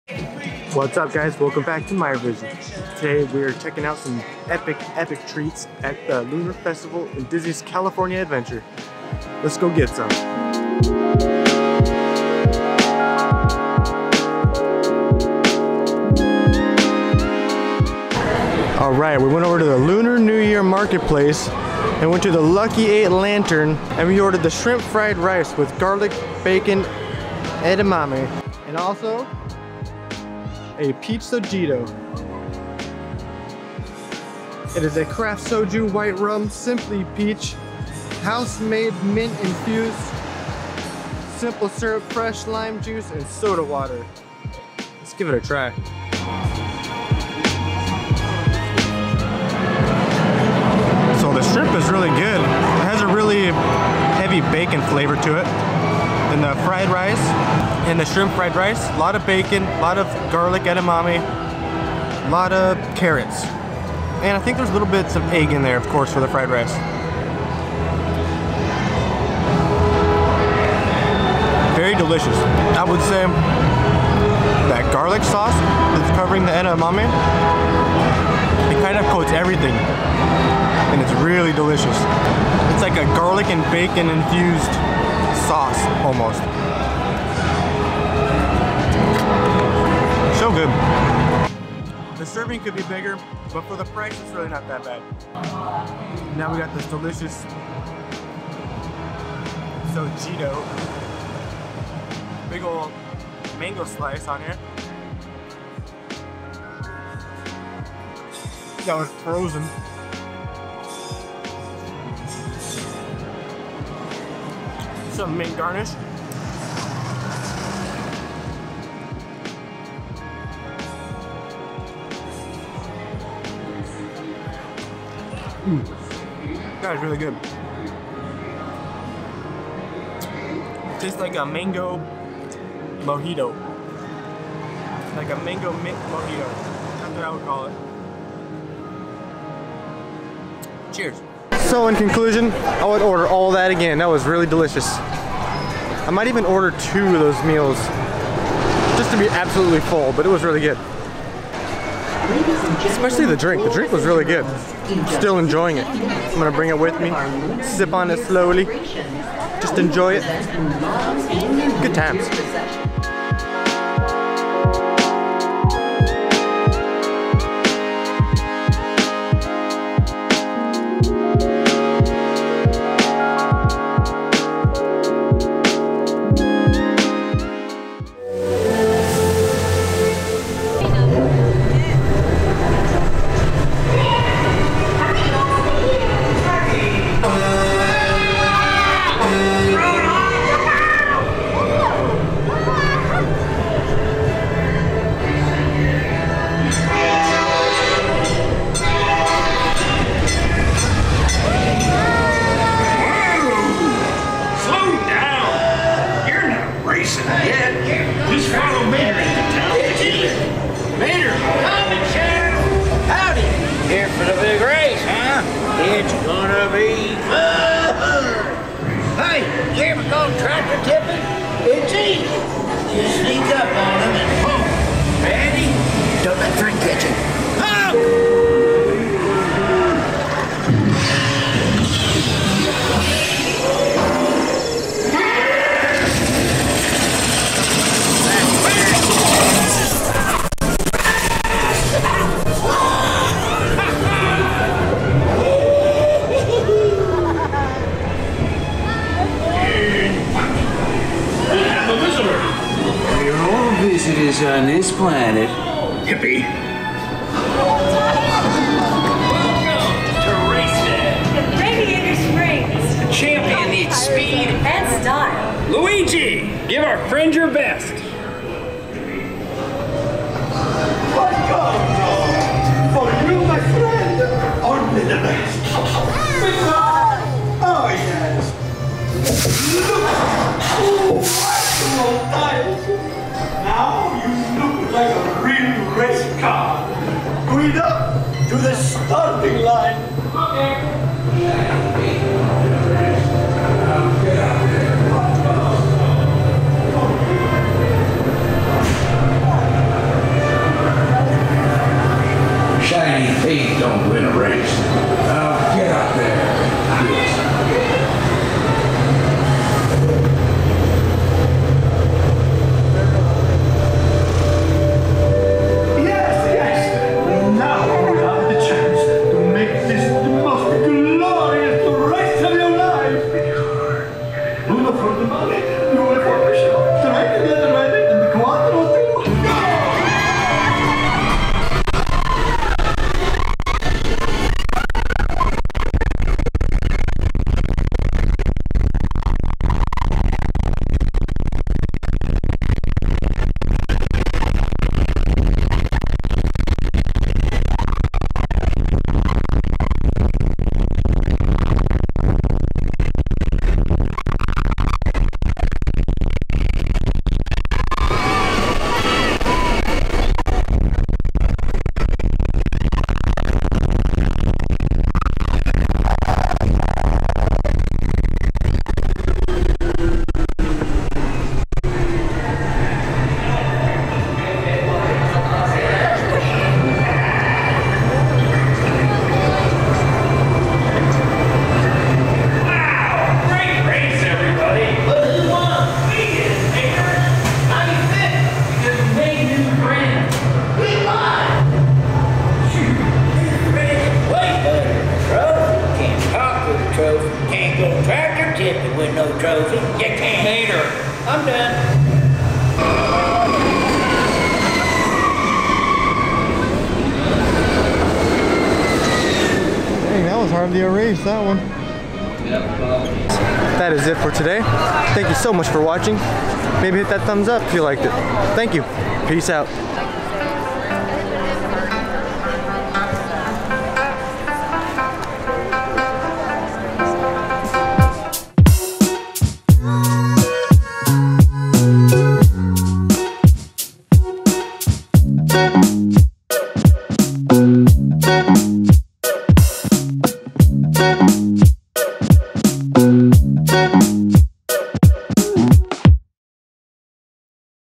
Trying Lunar New Year Food/Drink! Radiator Springs Racers! California Adventure!
Hey!!! In todays video we try out a meal from the Lucky 8 Lantern as part of the Lunar New Year Festival taking place at Disney's California Adventure! The food is delicious and there is lots more to try! We also take a ride on Radiator Springs Racers in Cars land! I hope you enjoy the video!